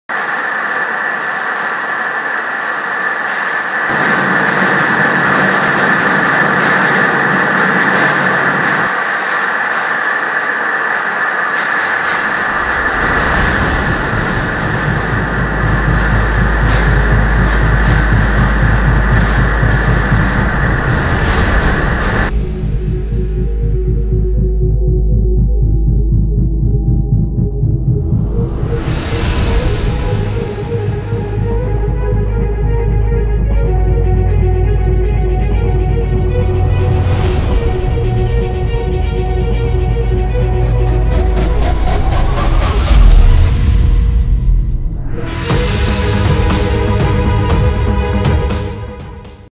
The Rashid Rover's Vibration Test
The vibration test on the Rashid Rover, the first Emirati mission to the Moon, was successfully completed by the Emirates Lunar Mission team at the Electro-Optics Center of Excellence facilities (EOCE), part of the Emirati-based leading advanced technology group EDGE.

The purpose of the test is to ensure that the rover's structure and electronics can withstand the stress of vibration loads during the journey to the Moon.